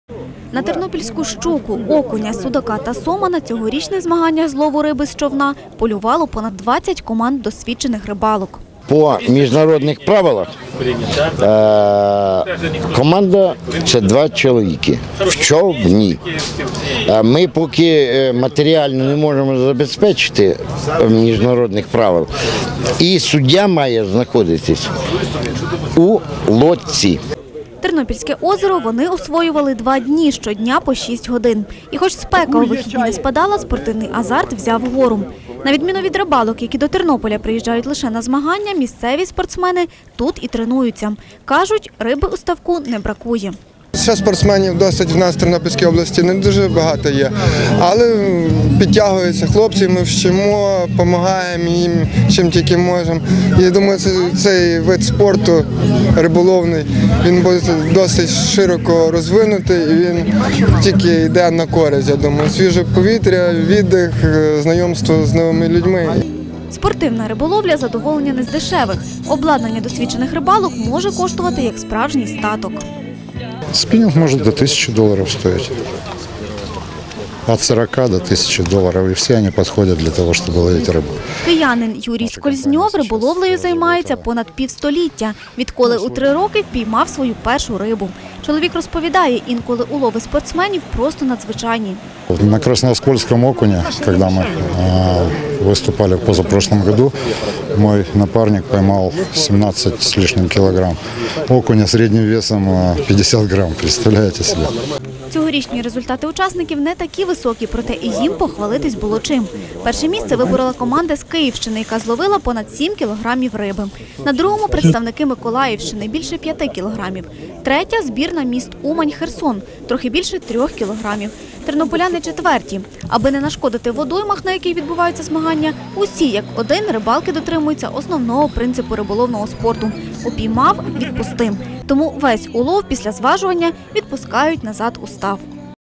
Кубок Західного регіону з лову риби з човна (Тернопіль)
На Тернопільському ставі вперше провели Кубок Західного регіону України з лову риби з човна. За нього боролись понад два десятка команд з 9 областей України. Змагання проходили два дні у два тури. Під час кожного з них спортсмени ловили рибу протягом шести годин. За підсумками уловів і визначили переможців.